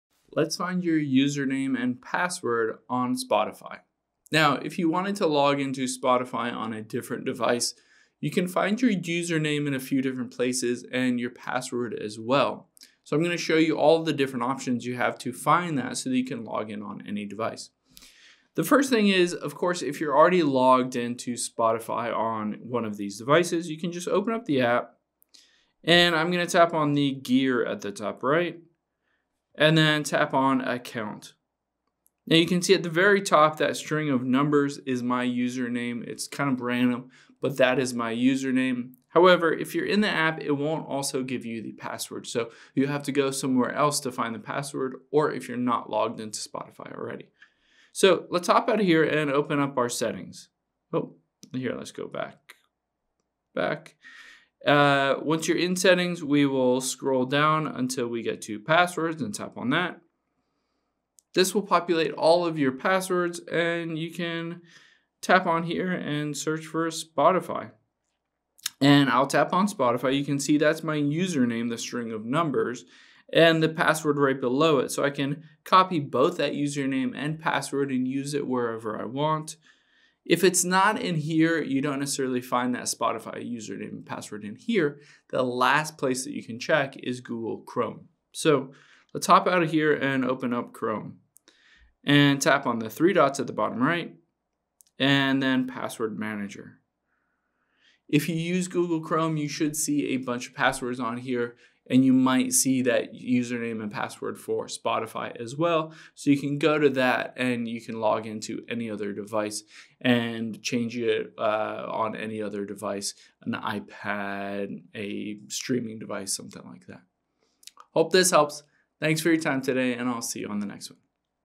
How To Find Your Username And Password On Spotify
Let's find both your username and your password on Spotify so you can login on another device or log back in if you forgot it.